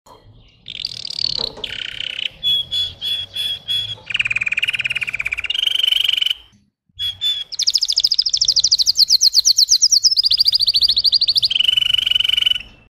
My red headed canary
Singer Canary. Teach them to sing like never before!
Autor: Akhad

HISTORY
History of Walnuts Russia has started three centuries ago. It is very unfortunate that the Kenari Russia is almost unknown in the world although this type of hobbyists claim that the type of modulation has the best walnut with songs adapted from the best birds native Russia.

Russian canary bred for singing. The song consists of 10-15 high tone variations and sung in 45-60 seconds. Variations in the Russian song, adapted from the original wild birds Russia, often named after a wild bird itself or a name similar to the variation of songs sung.

The following value and assessment sheets have been translated in English but still written in Russian also to avoid refraction of translation, as written in the official assessment form on the contest in Russia.